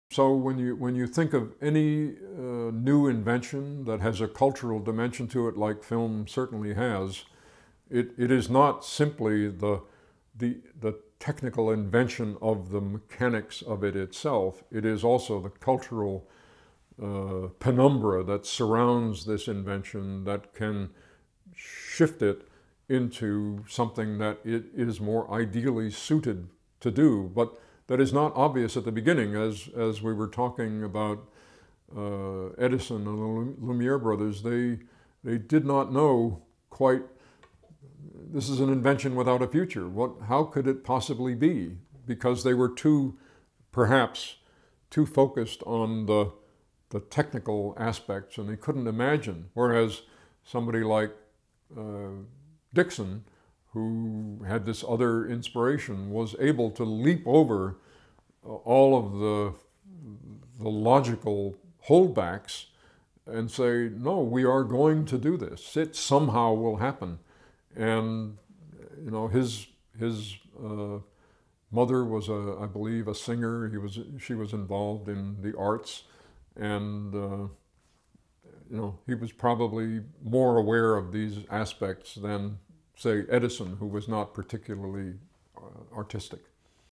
Walter Murch - The Three Fathers of Film: Thomas Edison (271/320)
Walter Scott Murch (b. 1943) is widely recognised as one of the leading authorities in the field of film editing, as well as one of the few film editors equally active in both picture and sound. [Listener: Christopher Sykes; date recorded: 2016]

TRANSCRIPT: So, when you think of any new invention that has a cultural dimension to it, like film certainly has, it is not simply the technical invention of the mechanics of it itself. It is also the cultural penumbra that surrounds this invention that can shift it into something that it is more ideally suited to do, but that is not obvious at the beginning.

As we were talking about Edison and Lumière brothers, they did not know quite... This is an invention without a future. What... How could it possibly be? Because they were too, perhaps, too focused on the technical aspects, and they couldn't imagine. Whereas, somebody like Dickson, who had this other inspiration, was able to leap over all of the logical holdbacks and say, 'No, we are going to do this. It, somehow, will happen.' And, you know, his mother was a, I believe, a singer. He was... she was involved in the arts. And, you know, he was probably more aware of these aspects than, say, Edison, who was not particularly artistic.